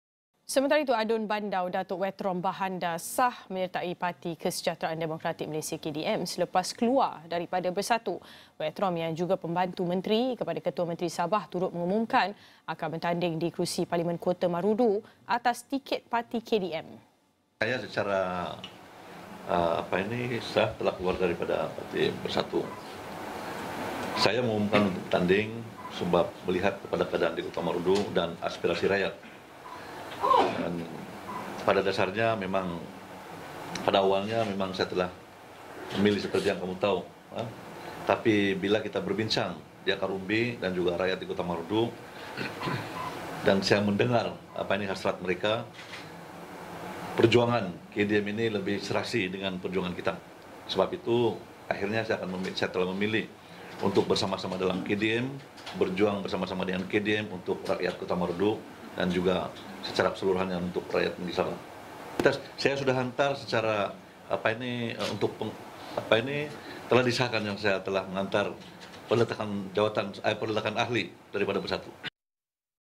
Wetrom sah sertai KDM, bertanding di Kota Marudu
ADUN Bandau, Datuk Wetrom Bahanda sah menyertai parti Kesejahteraan Demokratik Malaysia (KDM) selepas keluar daripada Bersatu.

Wetrom yang juga pembantu menteri kepada Ketua Menteri Sabah turut mengumumkan akan bertanding di kerusi Parlimen Kota Marudu atas tiket parti KDM.